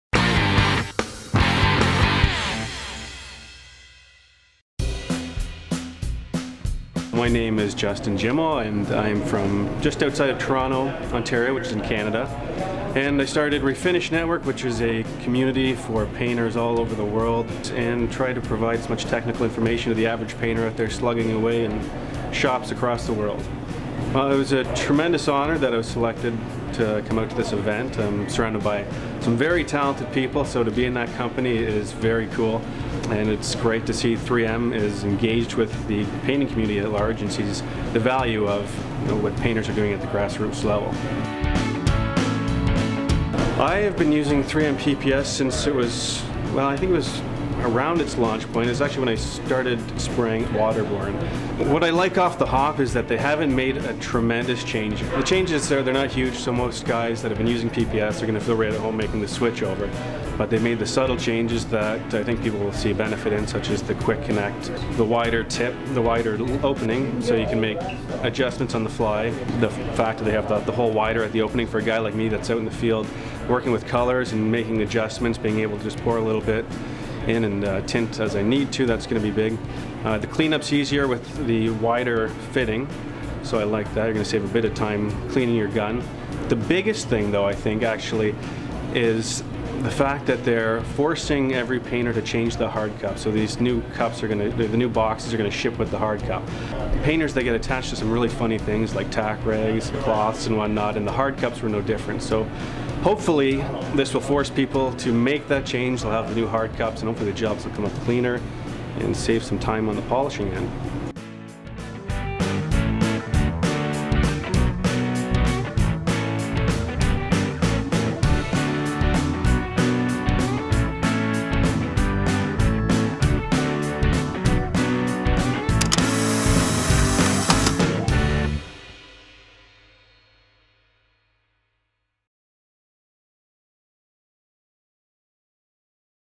3M™ PPS™ World Cup - Justin Jimmo
The 3M™ PPS™ World Cup in Detroit, MI highlighted some of the worlds best painters in our industry. Check out what Justin Jimmo had to say about his experience.